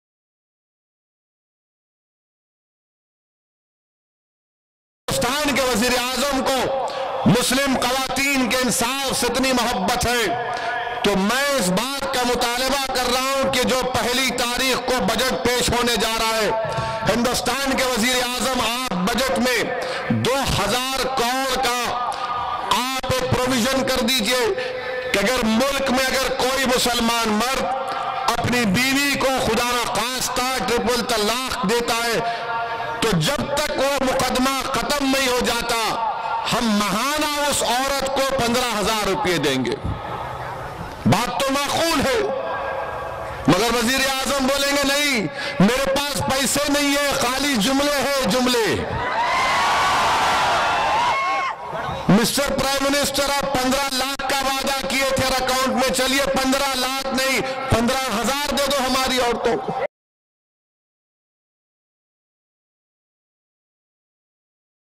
Owaisi demands monthly allowance for triple talaq victims - Telangana News
Sangareddy (Telangana), Jan 30 (ANI): All India Majlis-e-Ittehad-ul Muslimeen chief Asaduddin Owaisi criticised Triple Talaq Bill, which is expected to be discussed during the ongoing budget session and said the bill in current form was impractical, since the bill had no monetary provision for the victims of triple talaq practice.